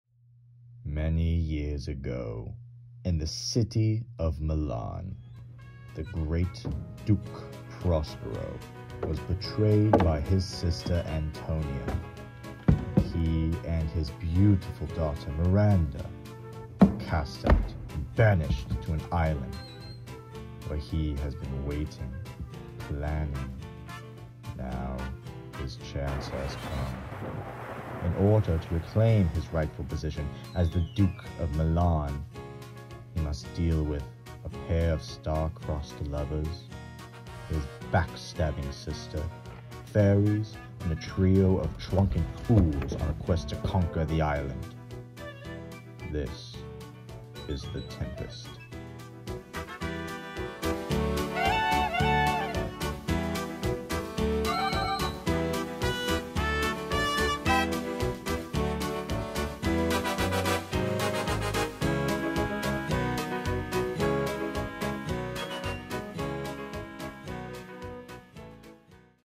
Johnson Fine Arts/ Theatre Presents: Tempest Trailer
Come join us for The Tempest – a tale of magic, power, and justice and what is said to be William Shakespeare’s final bow as a playwright.  The overthrown Duke of Milan, Prospero, who has been sequestered on a magical island sees his chance for revenge when a boat containing his overthrowers gets caught in a magical storm of Prospero’s making.

See what happens as he manipulates the nobles from whom he seeks revenge, a duo of fools who, with Prospero’s fish-man servant, get involved in hijinks galore, and the King’s son Ferdinand who begins to fall in love with Prospero’s own daughter Miranda. The Tempest shows October 7th – 9th at 7PM and October 10th at 2PM.